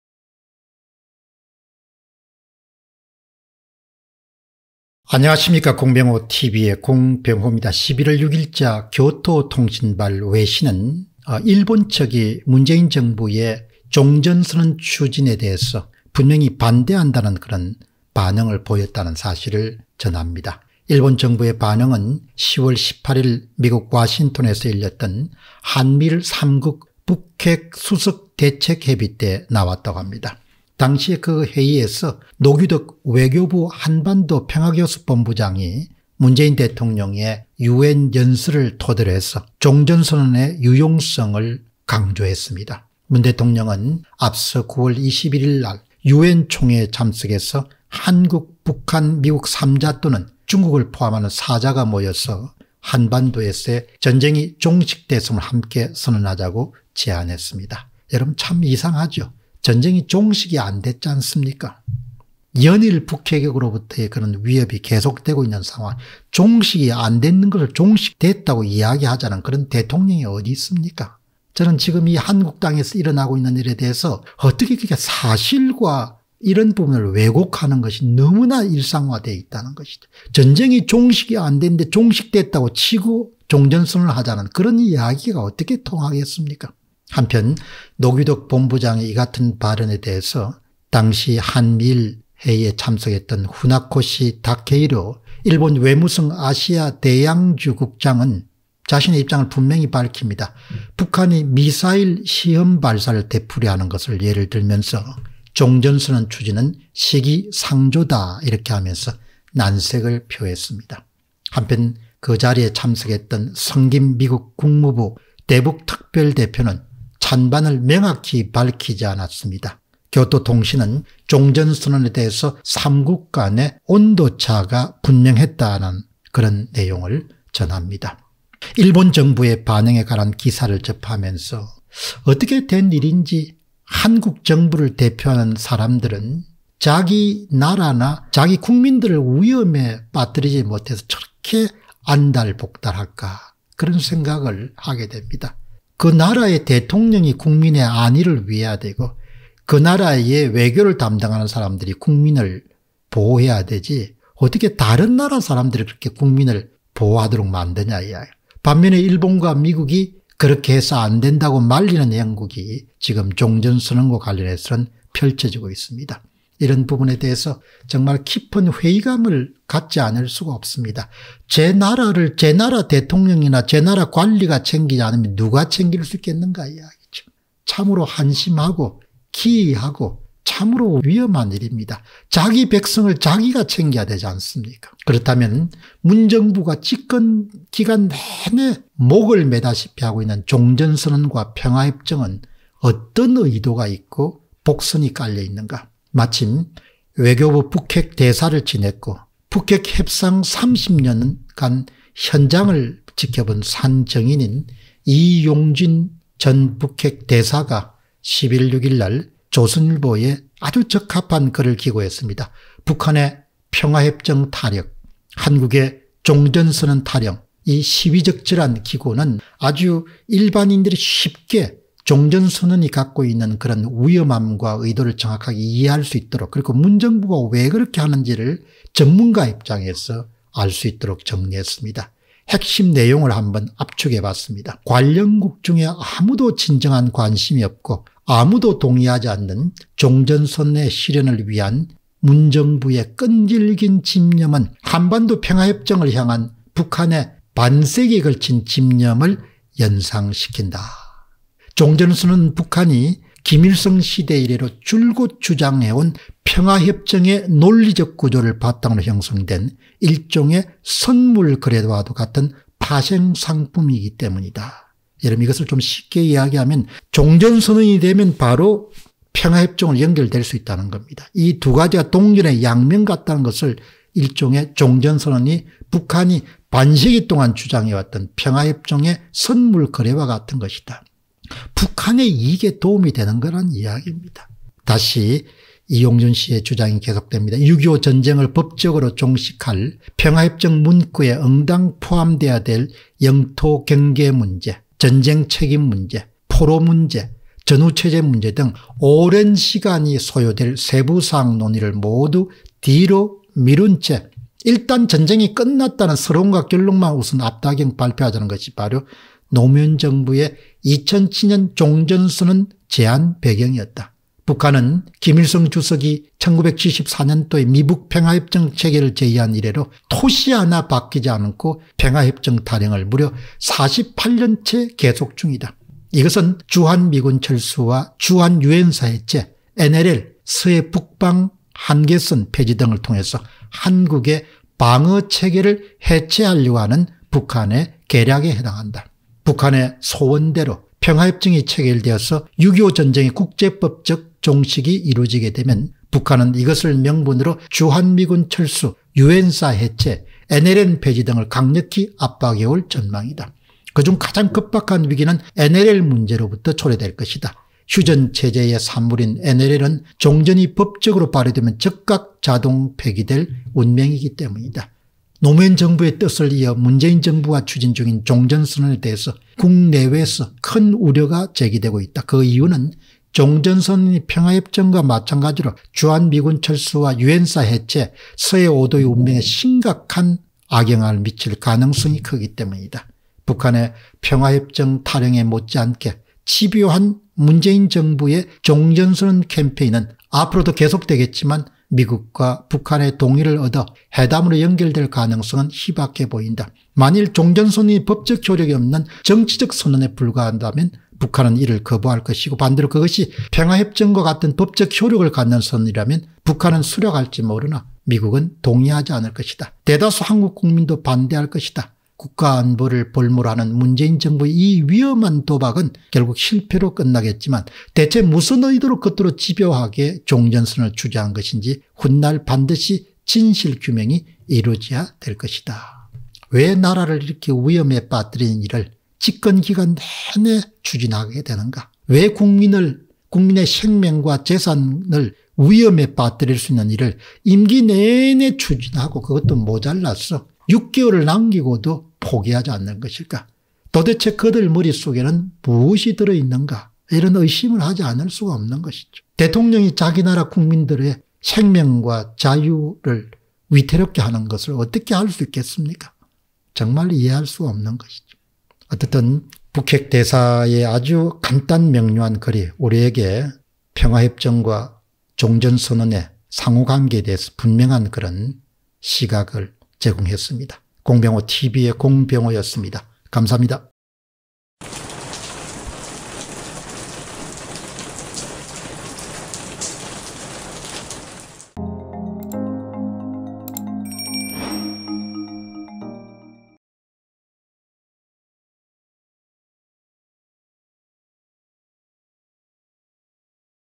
왜,자기 백성 생각을 먼저 하지 않을까 / 일본 종전선언에 부정적인 반응 피력 [공병호TV]
Bitcoin : 3MTW849AkP2ftLeSK2teZHMmzLu7Z83s9U